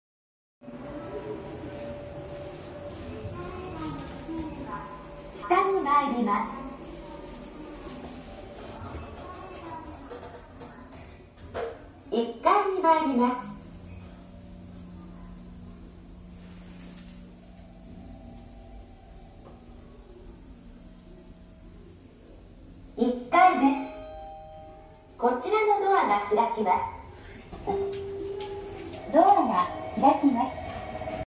【4K】門司駅北口エレベーター
門司駅の北口には2階がありませんでした。